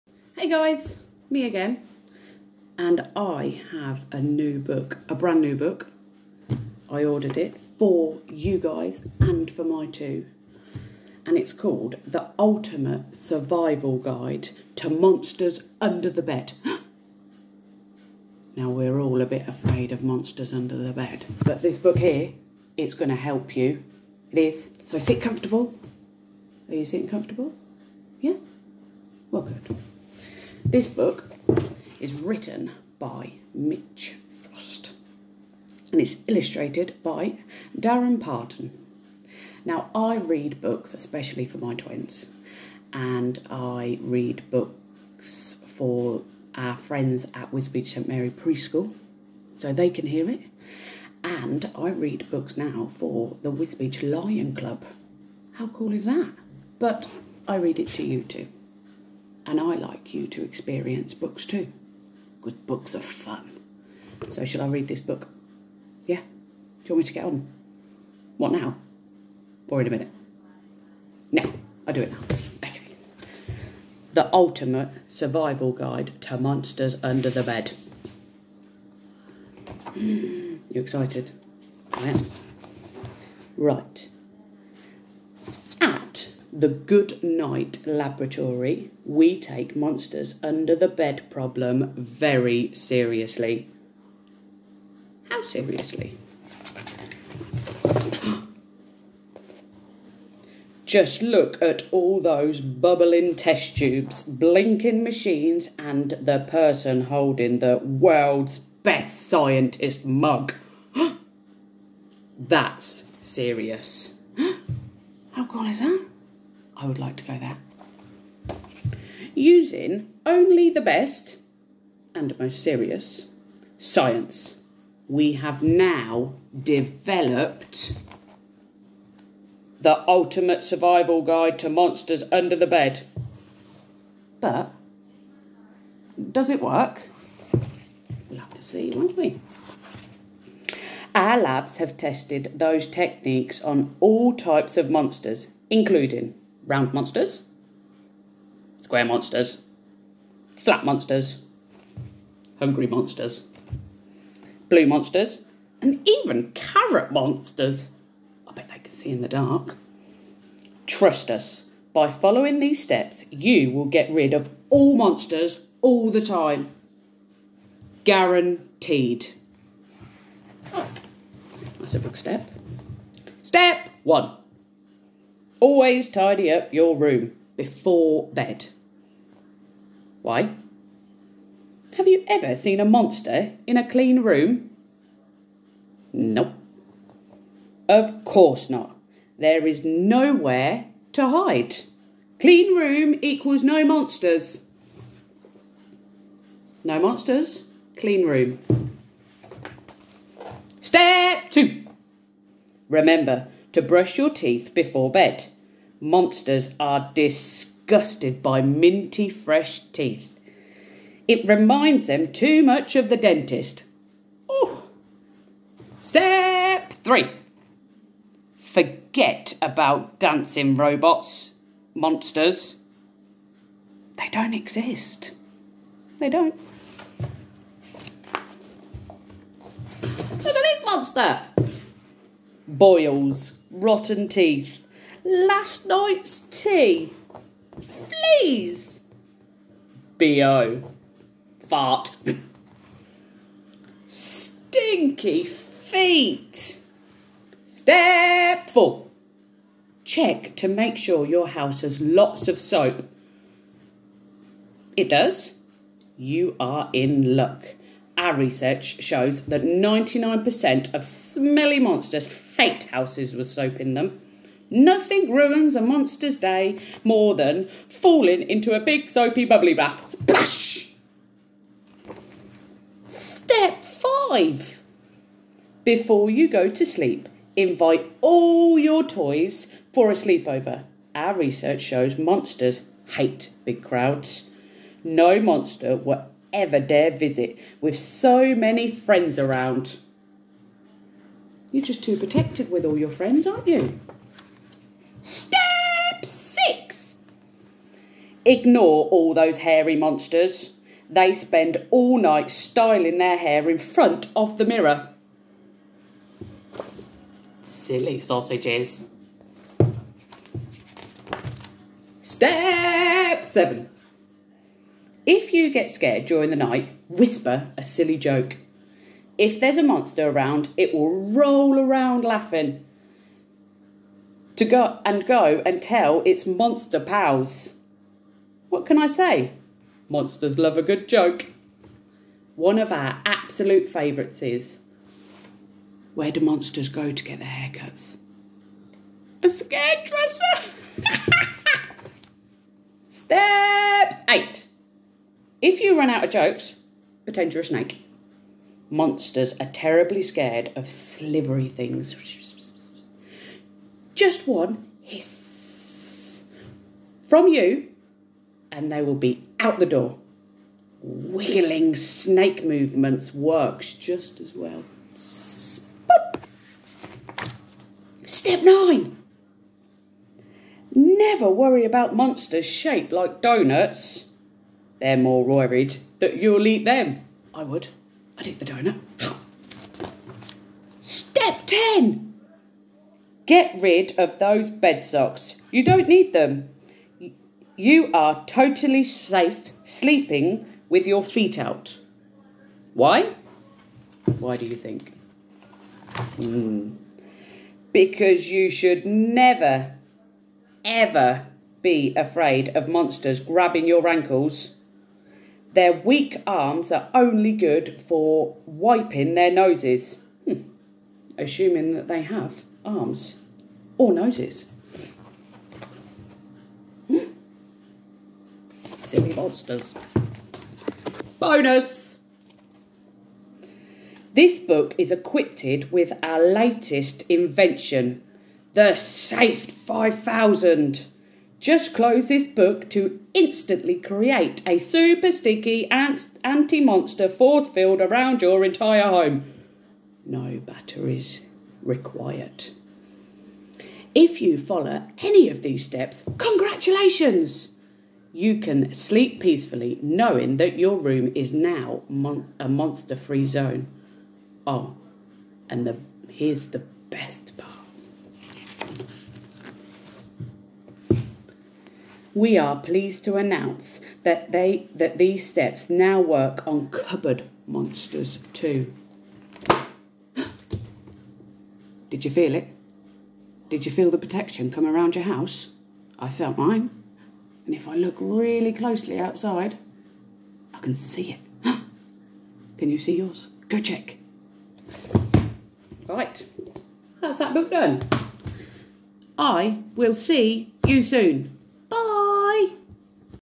The Ultimate Survival Guide To Monsters Under The Bed.
This book is written by Mitch Frost and illustrated by Daron Parton
And red by - Me, Mel 😀